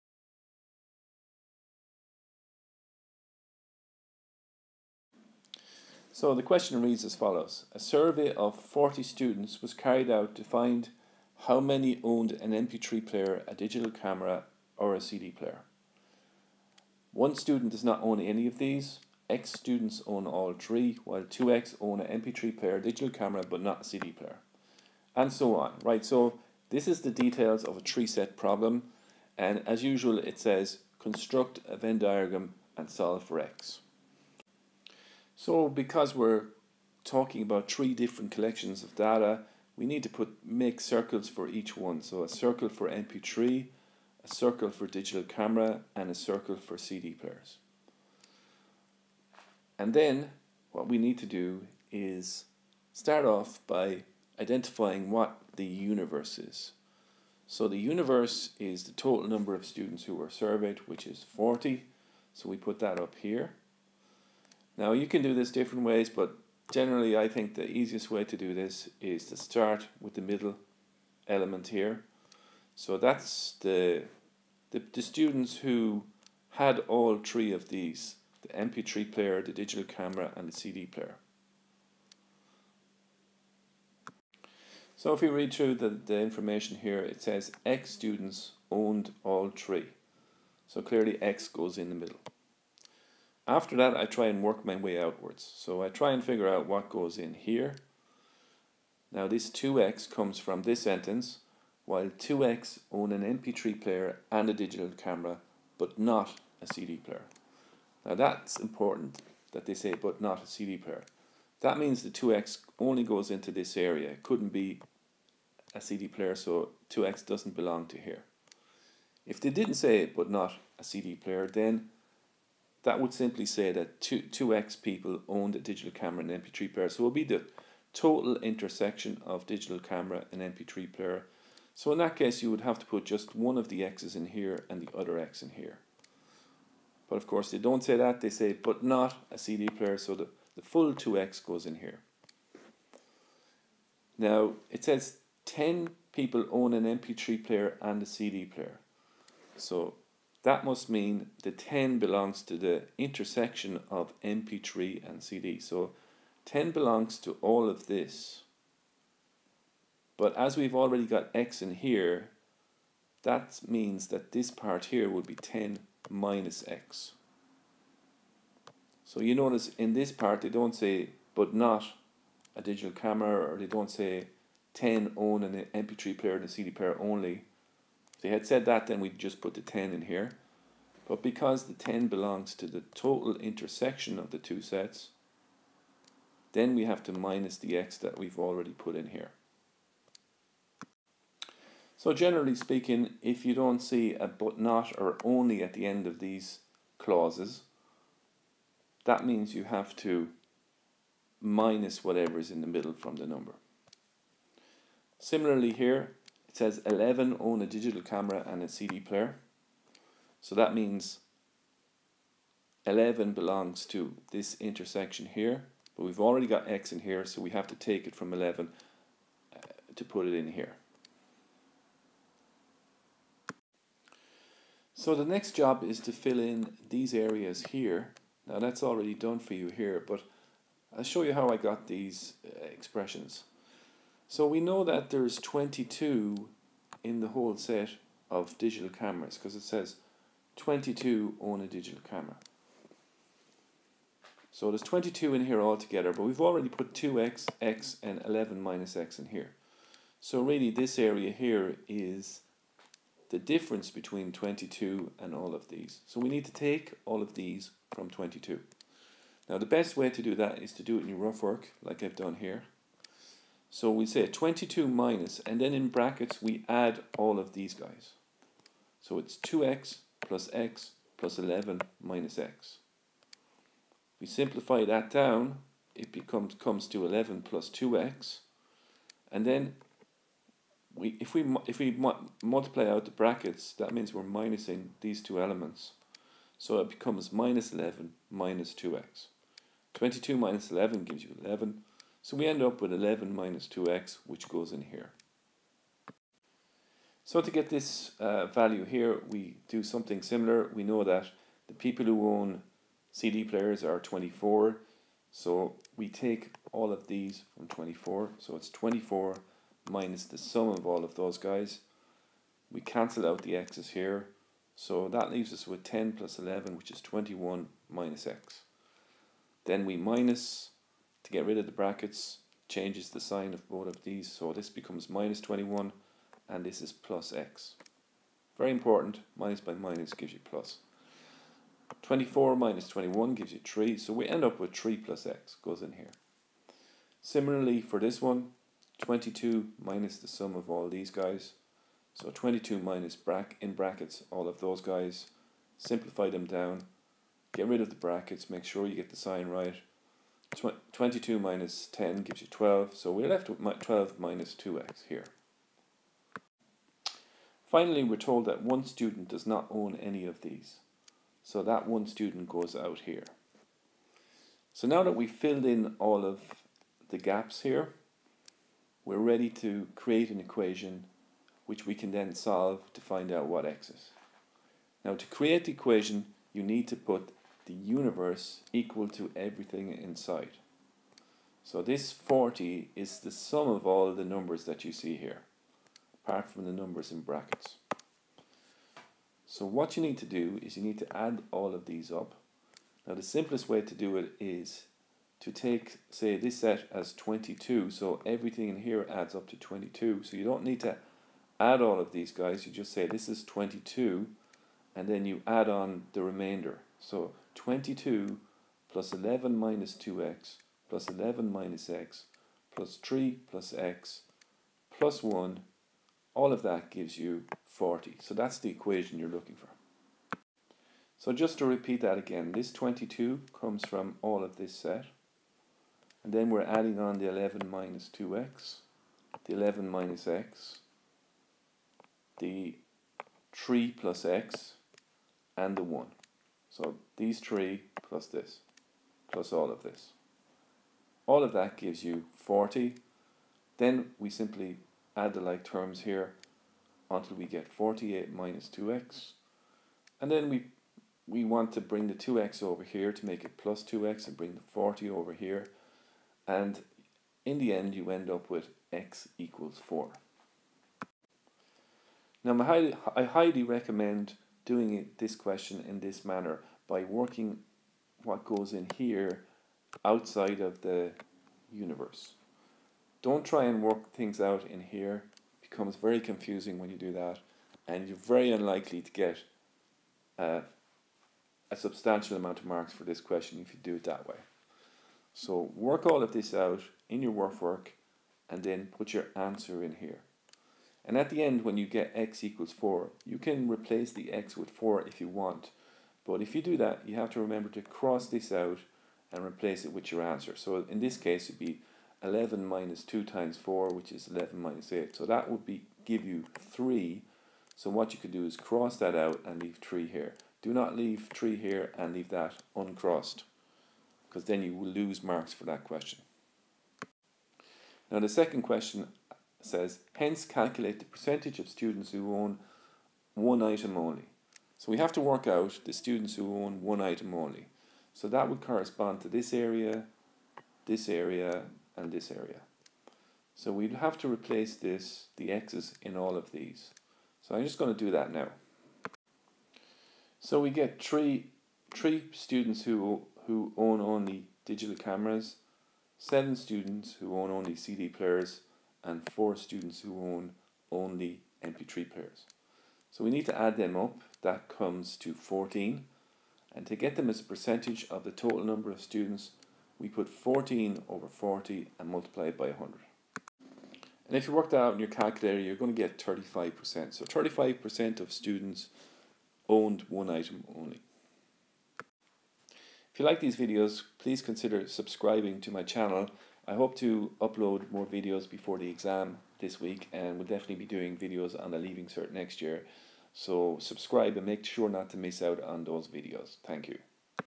Junior Cert Maths - 3 Set Venn Diagram
Tricky 3 set Venn diagram problem for junior Cert maths explained.